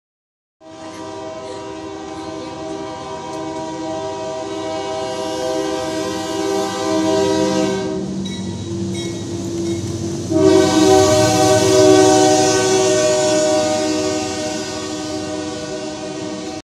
Evita accidente Chofer VS Tren / Via del Ferrocarril Puebla
Es la madrugada del 18 de agosto de 2017, el Chofer alcanza a frenar justo a tiempo y evita accidente / Maquinista toca tarde el silbato en cruce de via de Ave Hermanos Serdán en Puebla. Uffff